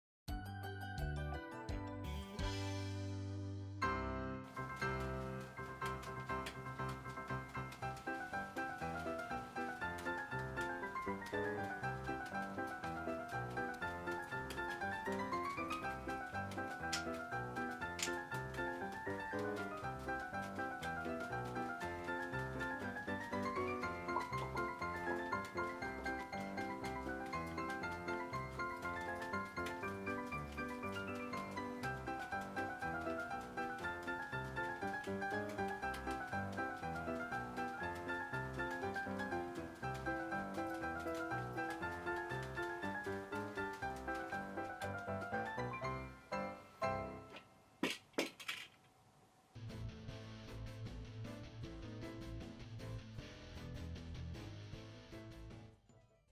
Juggling 5 Balls in Style!
This 5 ball juggling pattern is transforming my fashion sense. It's a good thing, I could probably use a little extra.